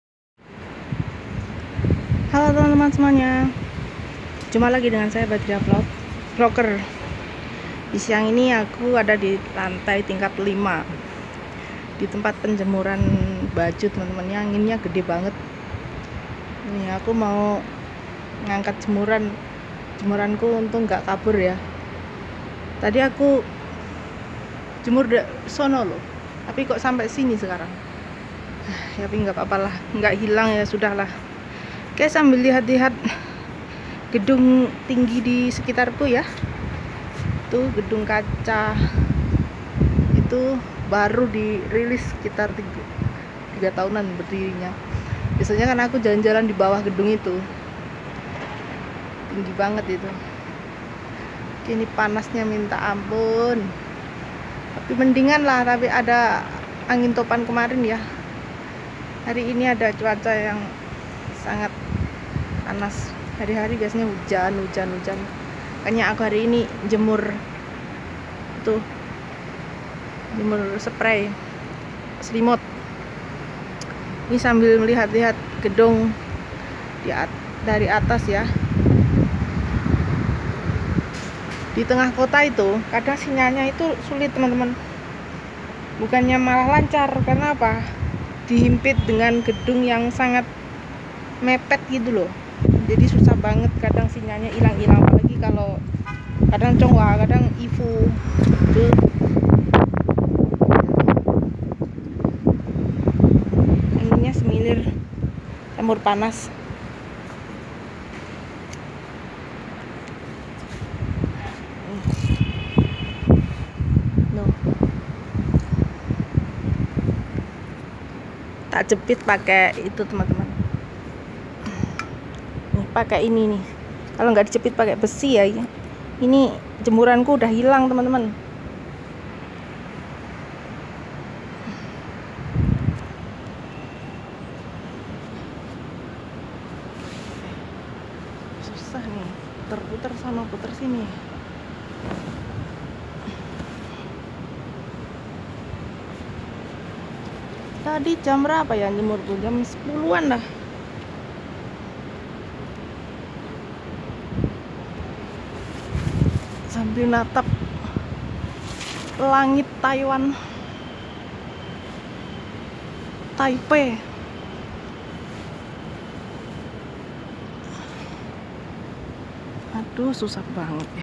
Lihat jemuran d tingkat lima Taiwan, berantakan d terpa angin kencang
Badriyah Vloger#Taiwan#Taipei Nanjing Fixing.